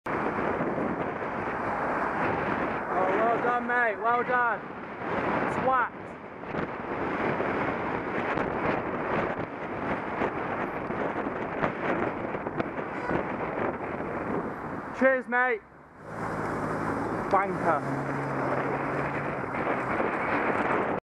B16RYO - No Observation Whatsoever
Who knew that overtaking a cyclist and a parked car on a dual carriageway could be so monumentally difficult?